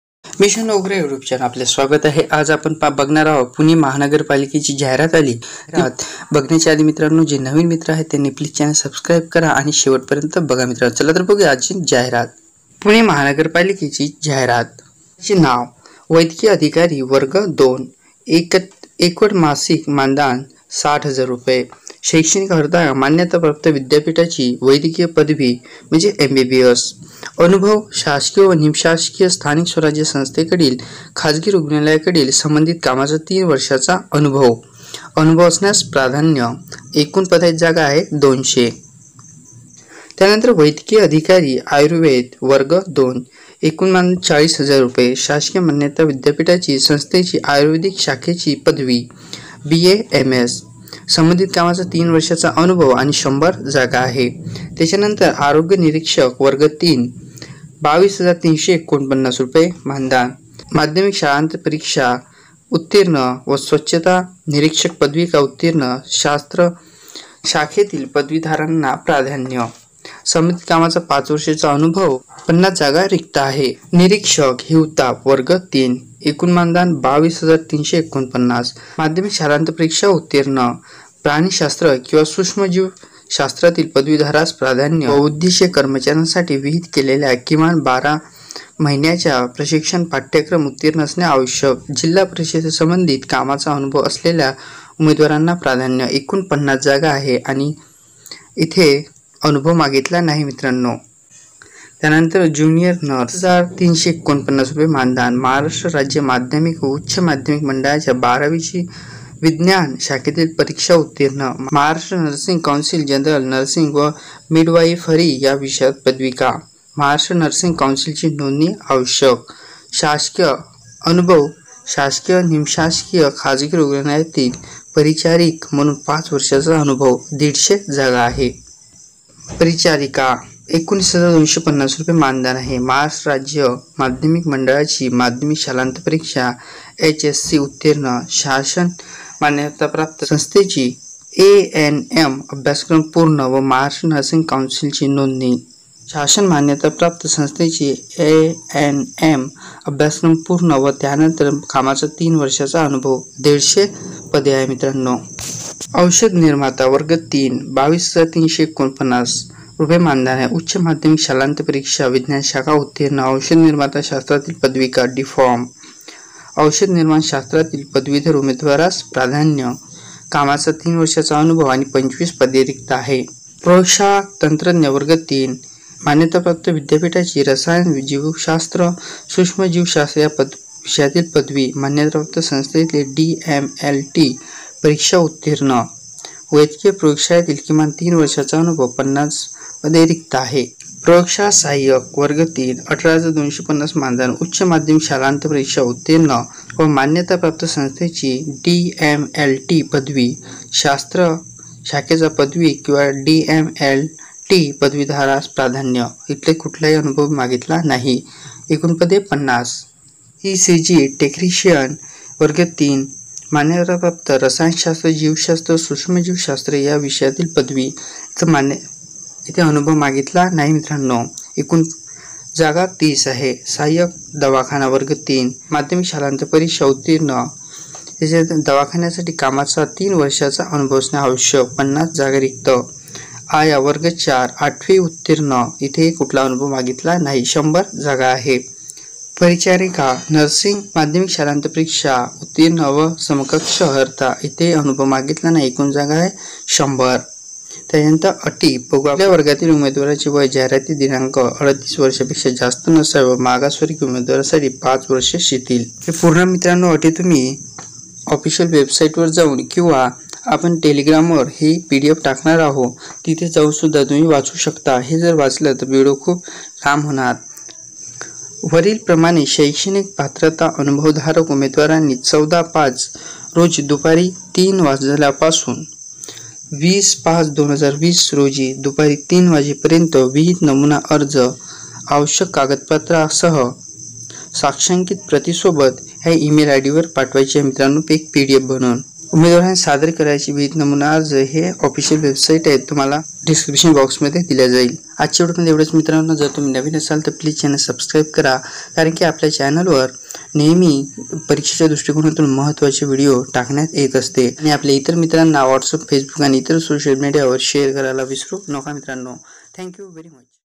(PMC) पुणे महानगरपालिकेत 1105 जागांसाठी भरती / job
(PMC) पुणे महानगरपालिकेत 1105 जागांसाठी भरती

अर्ज कसा करावा: अर्जाची प्रिंट काढून अर्ज भरावा व आवश्यक कागदपत्रांच्या प्रती PDF फॉरमेट मध्ये तयार करून संबंधित ईमेल आयडी वर पाठवा